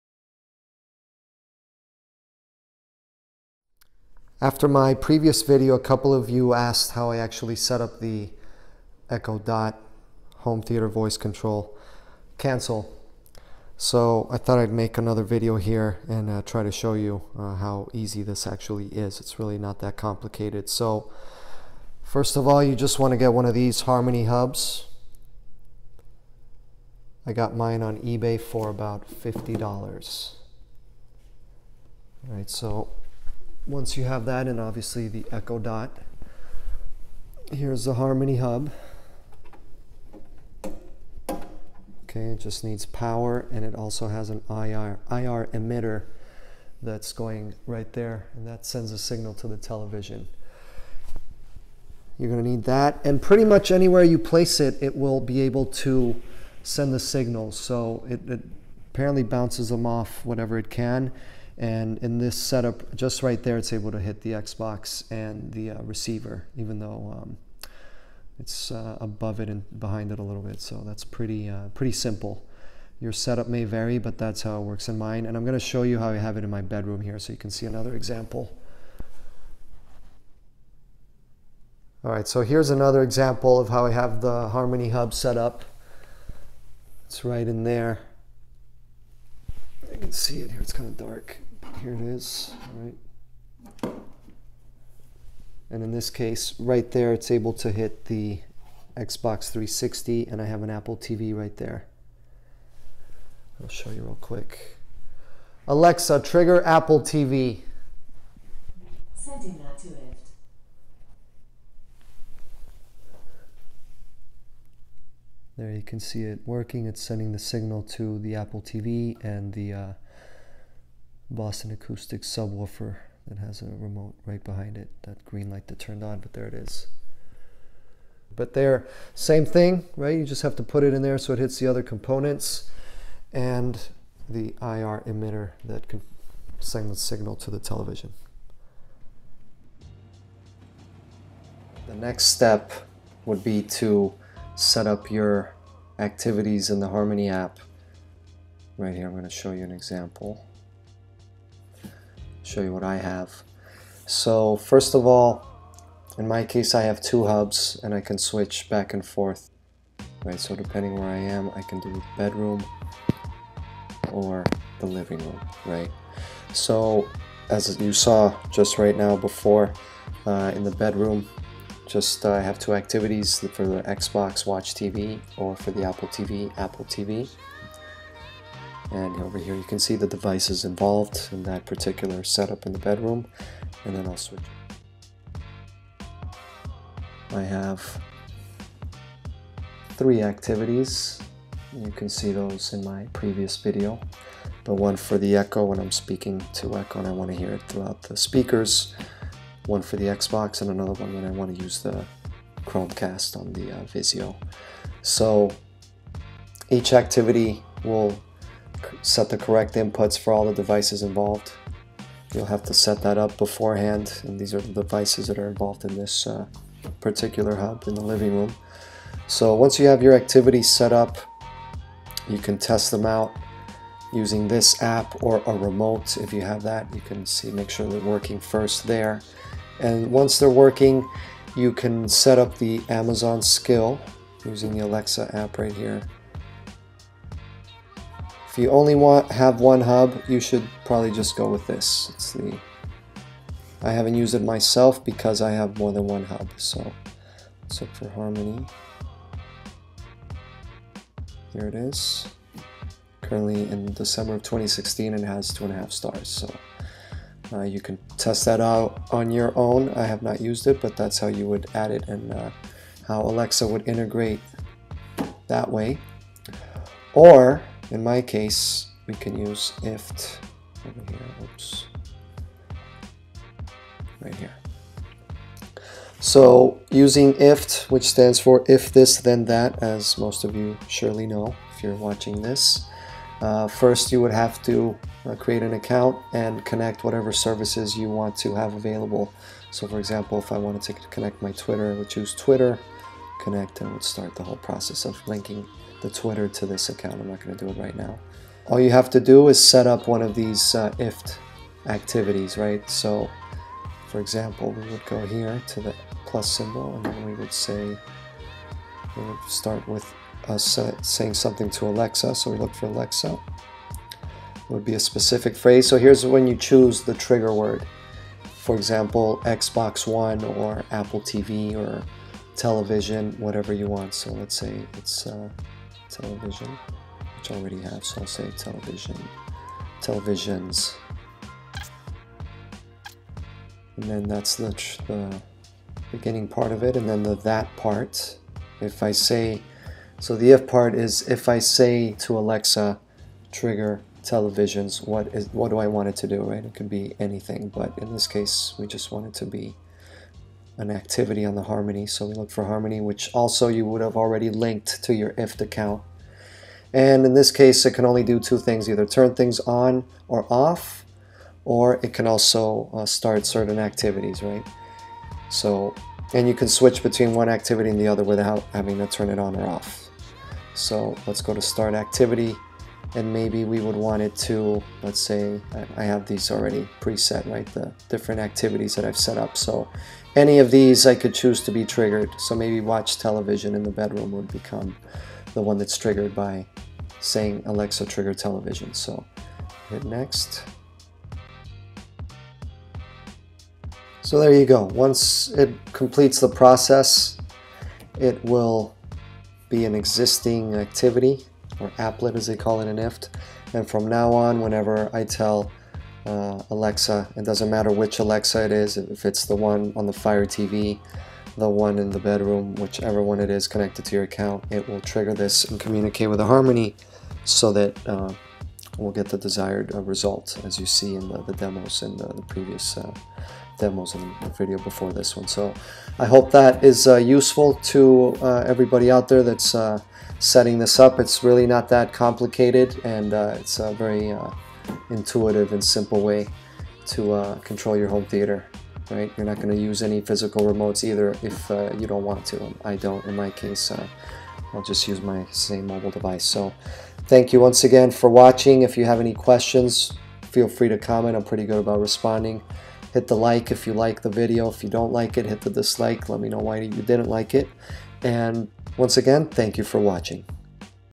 Amazon Alexa, Logitech Harmony Hub and IFTTT - Setup Guide
A guide for setting up Amazon Alexa to voice control your home theater using a Harmony Hub and IFTTT.
Logitech Harmony Hub

Vizio E55-E2

Amazon Echo Dot

Phillips Hue LED Light Strip